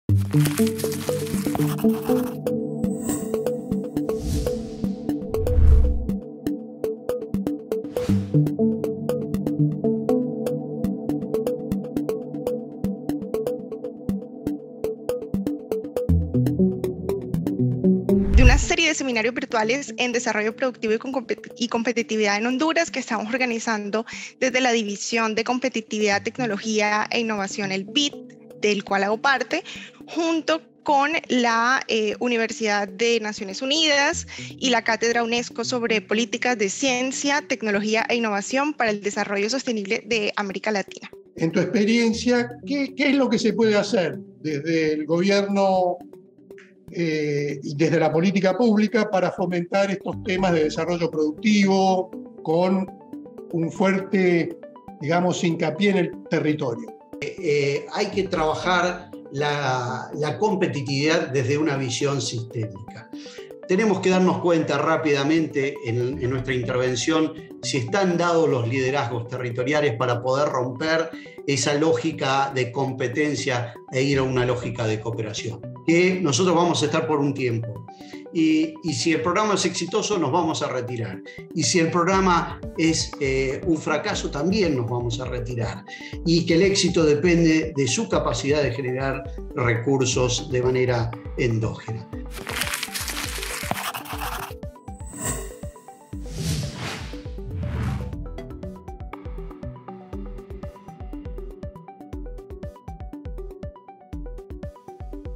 Programas asociativos para fomentar clústeres y aglomeraciones productivas de base agrícola
Experiencias y lecciones de programas asociativos para fomentar clústeres y aglomeraciones productivas de base agrícola.
En este webinar realizado el 14 de octubre de 2022, exploramos experiencias recientes y lecciones para fomentar la asociatividad en clústeres y aglomerados productivos de base agrícola y agroindustrial. También analizamos algunos casos con grupos asociativos del PROSAP (Programa de Servicios Agrícolas Provinciales de Argentina) donde se plantea, por ejemplo, sobre qué actividades trabajar, cómo seleccionar grupos asociativos y sectores, cómo combinar liderazgos públicos con iniciativa privada, entre otros temas de interés para el fomento de este tipo de actividades.

Este es el tercero de una serie de 5 webinars en desarrollo productivo y competitividad en Honduras organizado por la División de Competitividad, Tecnología e Innovación del BID en alianza con la Universidad de Naciones Unidas UNU-MERIT y la Cátedra UNESCO sobre “Políticas de Ciencia, Tecnología e Innovación para el Desarrollo Sostenible de América Latina”.

Invitado especial: Marcelo Yangosian, Jefe de la Unidad de Desarrollo Competitivo de la Secretaría de Agricultura, Ganadería y Pesca de Argentina. Licenciado en Economía Política de la Universidad de Buenos Aires (UBA) y Máster en Finanzas de la Universidad Torcuato Di Tella en Argentina.

Moderador: Carlo Ferraro, consultor, asesor y docente en temas relacionados con políticas de apoyo a MiPymes, programas asociativos y competitividad territorial. Actualmente da clases en las Maestrías en Desarrollo Territorial de la Universidad Tecnológica Nacional (UTN) Sede en Buenos Aires, y participa en el diseño y dictado de clases de una nueva Maestría en Gestión de Política Públicas que se está dictando en forma virtual en la Universidad de San Marcos en Perú. Ha sido recientemente asesor del Ministerio de Desarrollo Productivo y de la Secretaría de la Pequeña y Mediana Empresa (SEPYME) de Argentina. Ha trabajado durante casi tres décadas en la Comisión Económica para América Latina CEPAL con experiencia en asistencia técnica en varios de los países de la región. Doctor en Ciencias Económicas de la Universidad de Nanterre (Paris Nord), Francia.

**Encuentra más información sobre todos los webinars de esta serie